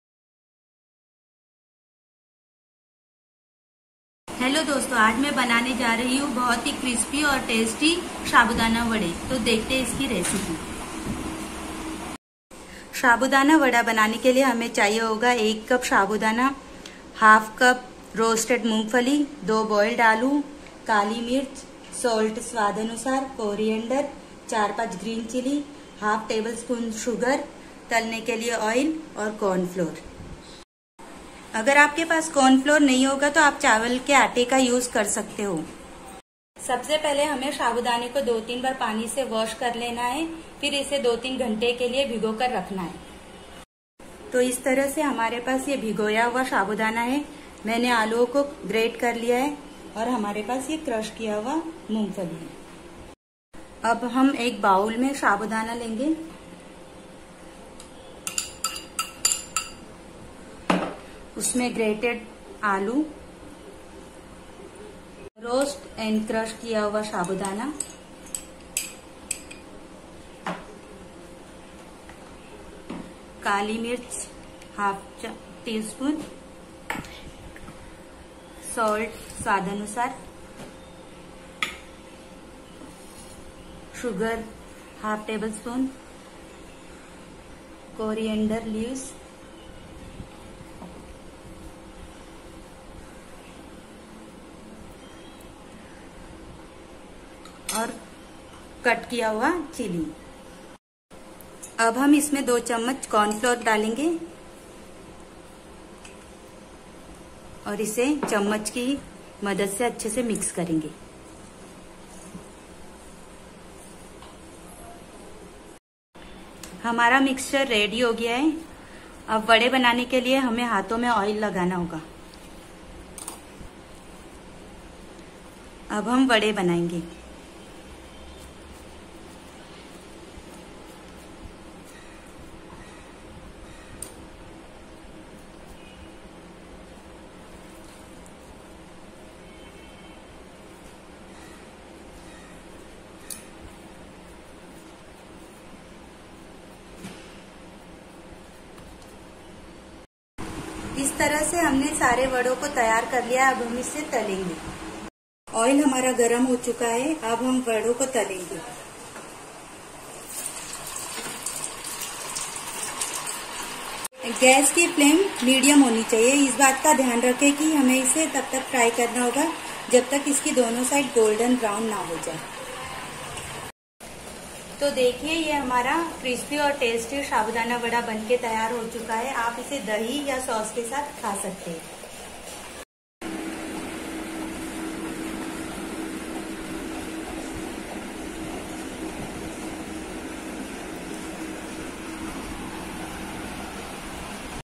How to make crispy, tasty and very easy shabudana vada.
Ingredients:
1.shabudana
2.boiled potatoes
3.green chilli
4.corriender
5.black pepper
6.salt
7.sugar
8.corn flour